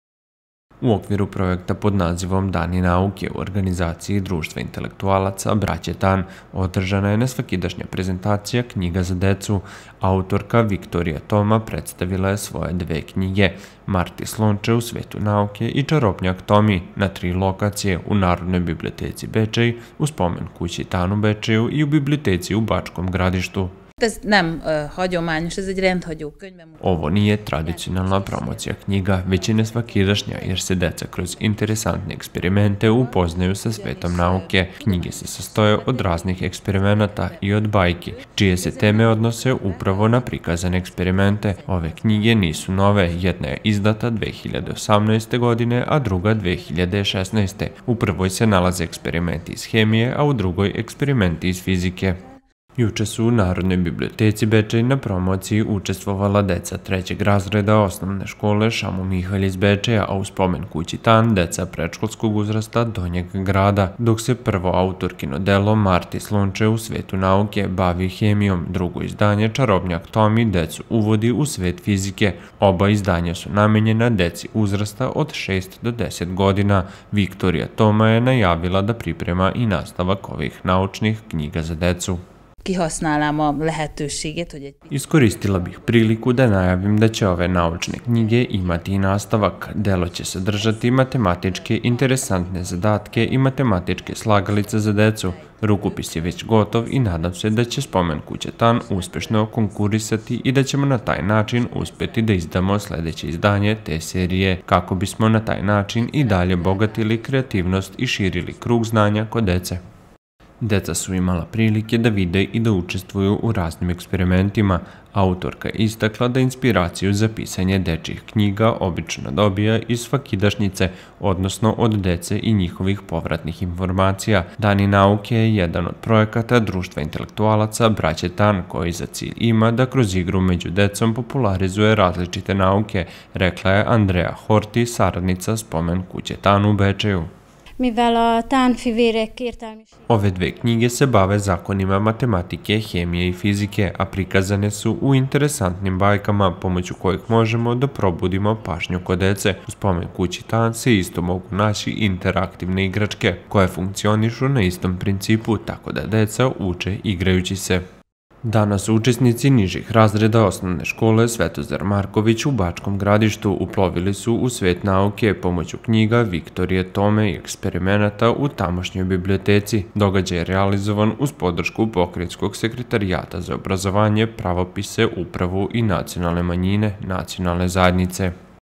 TV BEČEJ: „Dani nauke” Promocija dečijih knjiga sa interesantnim eksperimentima
TV BEČEJ: U okviru projekta pod nazivom „Dani nauke” u organizaciji Društva intelektualaca braće Tan, održana je nesvakidašnja prezentacija knjiga za decu. Autorka Viktorija Toma predstavila je svoje dve knjige „Marti, slonče u svetu nauke” i „Čarobnjak Tomi” na tri lokacije - u Narodnoj biblioteci Bečej, u Spomen kući „Tan” u Bečeju i u Biblioteci u Bačkom Gradištu.